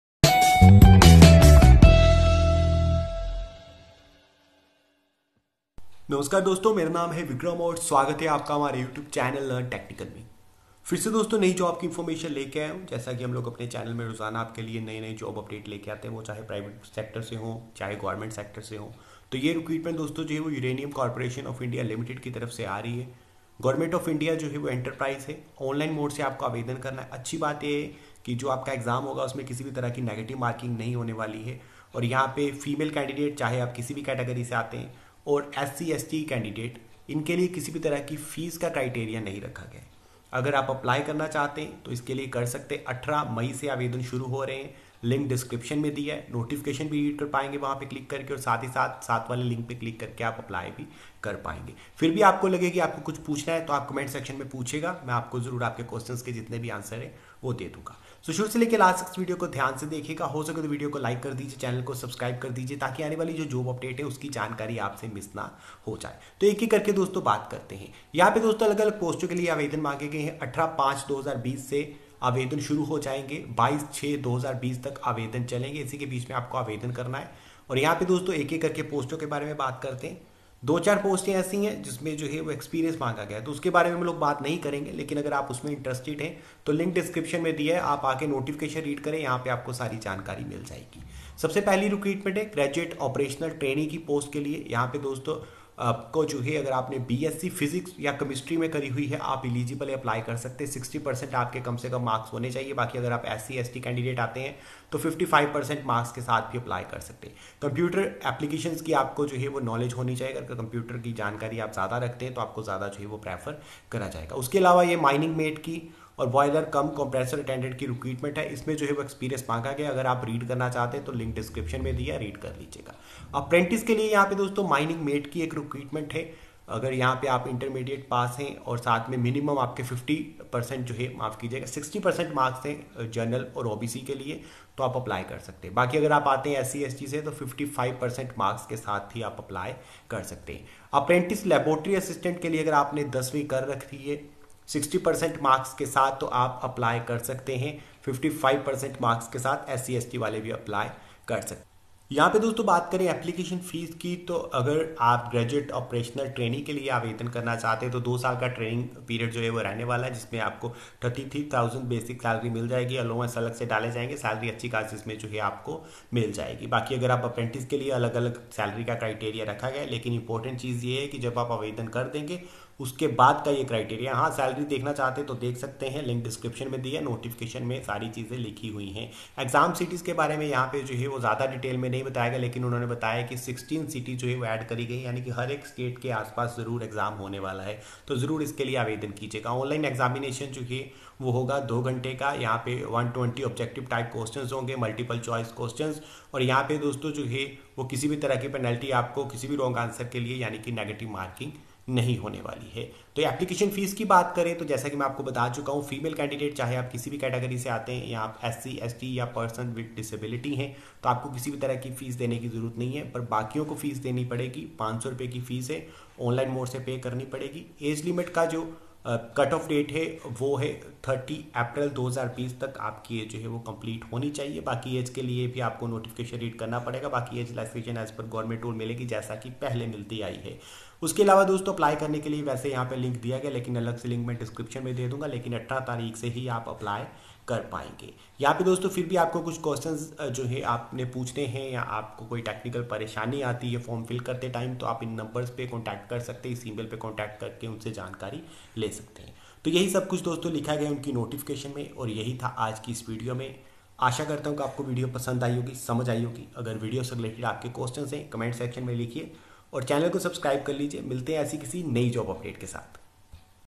Uranium Corporation of India Limited (UCIL) Recruitment 2020 I JOBS 2020
Uranium Corporation of India Limited (UCIL) Recruitment 2020

Uranium Corporation of India Limited (UCIL), a Public Sector Enterprise under the administrative control of the Department of Atomic Energy invites applications from suitable Indian Nationals UCIL invites applications from young, energetic and dynamic persons for recruitment THROUGH ONLINE MODE